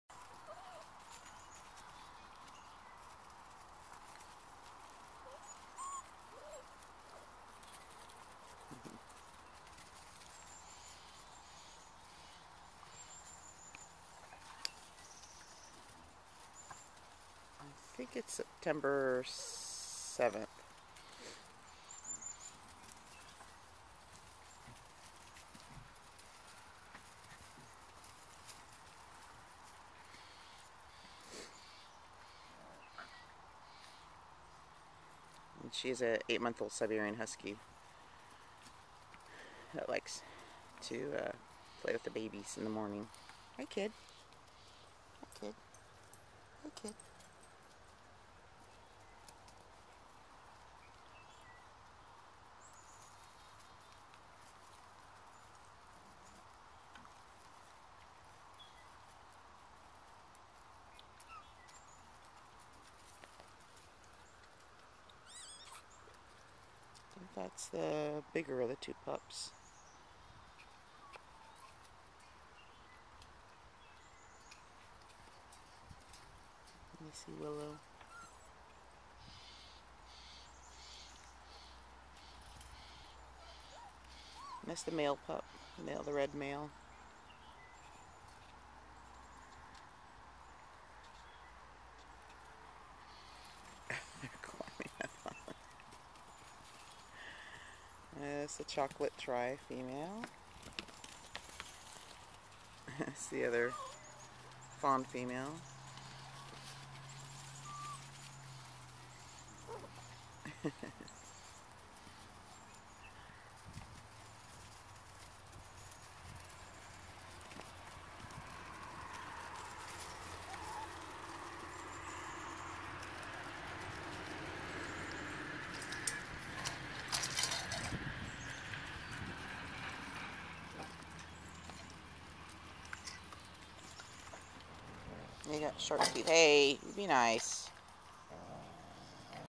Aura and pups 2
My Siberian Husky with Australian Kelpie pups (Tazo/Taurus Pups)